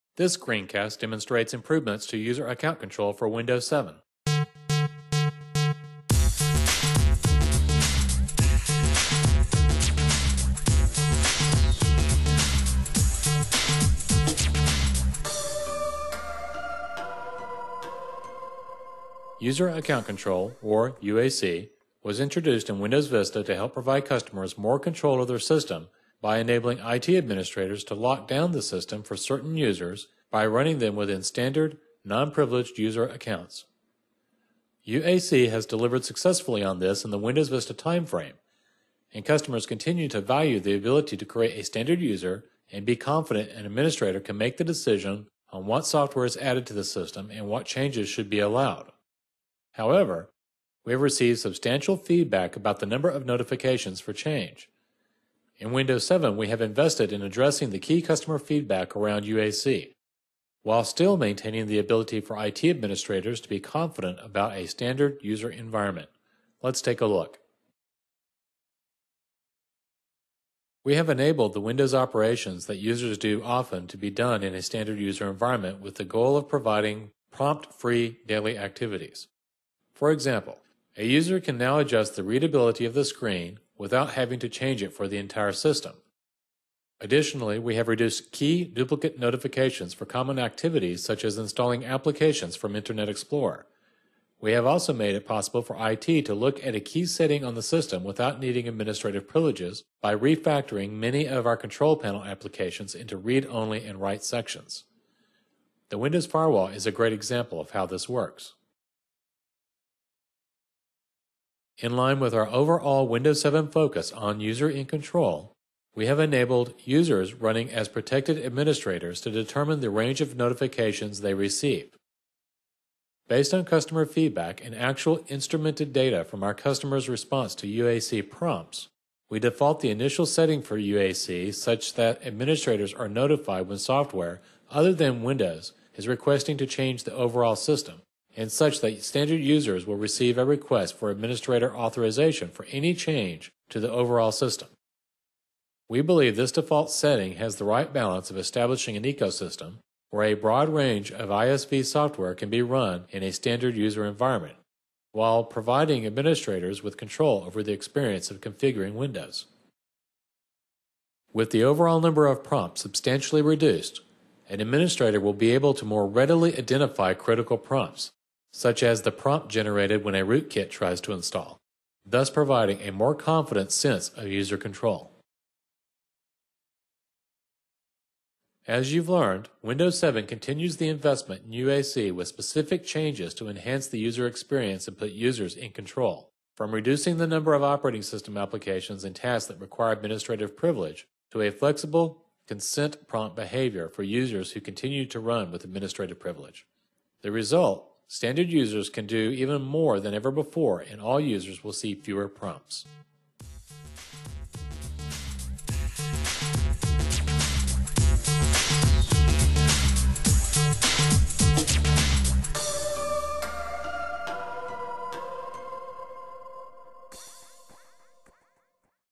Windows 7 - User Account Control
User Account Control (UAC) was introduced in Windows Vista to help increase security and improve total cost of ownership by enabling the operating system to be deployed without administrative privileges. See how Windows 7 continues the investment in UAC with changes that enhance the user experience and put users in greater control of their prompting experience. For example, the number of operating system applications and tasks that require elevation is reduced, so standard users can do more than before and will see fewer elevation prompts.